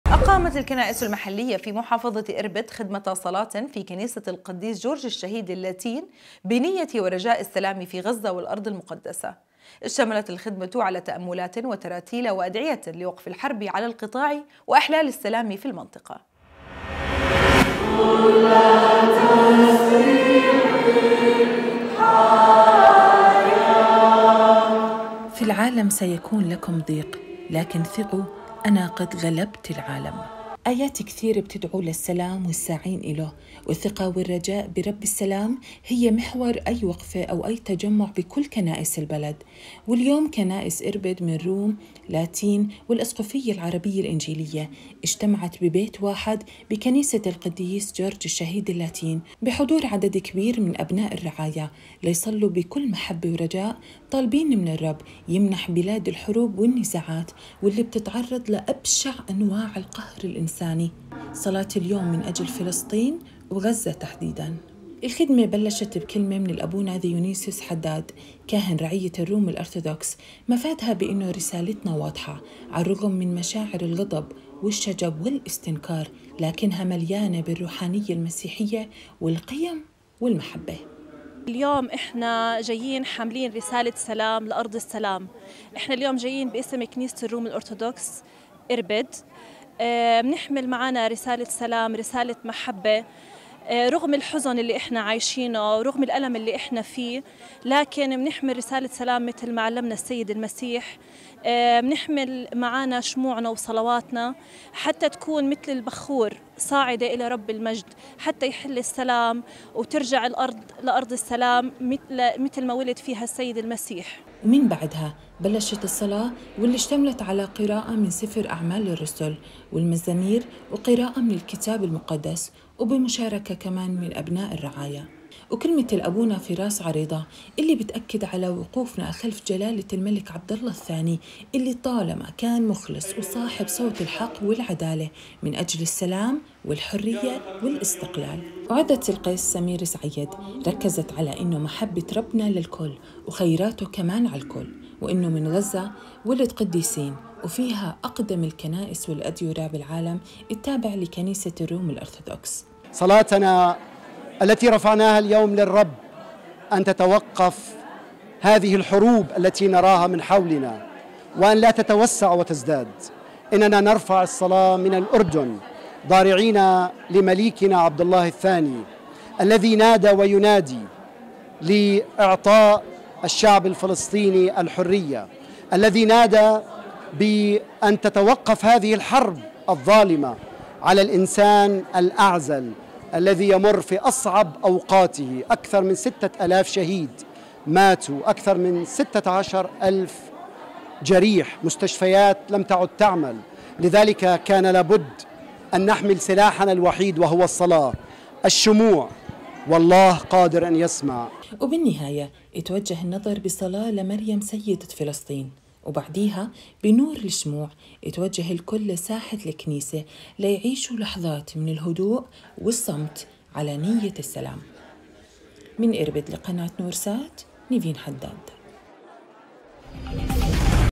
أقامت الكنائسُ محافظةِ اربد خدمةَ صلاةٍ في كنيسةِ القديس جورج الشهيد للاتين بنيةِ السلامِ في غزةَ
أقامت الكنائسُ المحليةُ في محافظةِ اربد خدمةَ صلاةٍ في كنيسةِ القديس جورج الشهيد للاتين بنيةِ ورجاءِ السلامِ في غزةَ والأرضِ المقدسه / اشتملت الخدمةُ على تأملاتٍ وتراتيلَ وأدعيةٍ لوقفِ الحربِ على القطاعِ وإحلالِ السلامِ في المنطقه